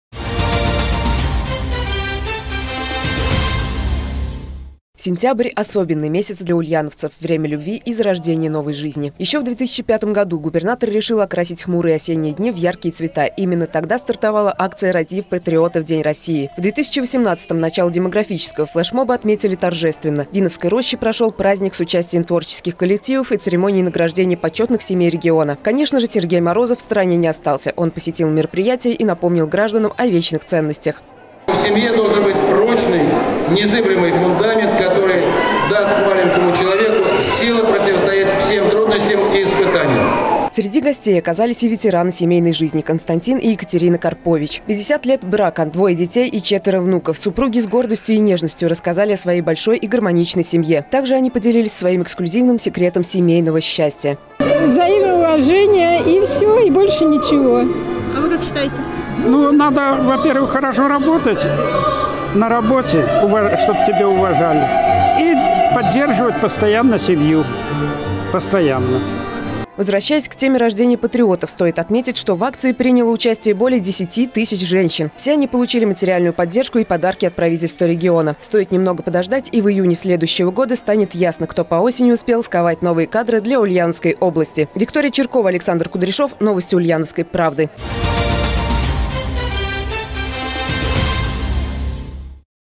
В Ульяновске начался месяц любви. В регионе стартовала акция «Роди патриота в День России»
Начало  демографического флешмоба отметили торжественно – в Винновской роще прошёл праздник с участием творческих коллективов и церемонией награждения почётных семей региона. Конечно же, Сергей Морозов в стороне не остался – он посетил мероприятие и  напомнил гражданам о вечных ценностях.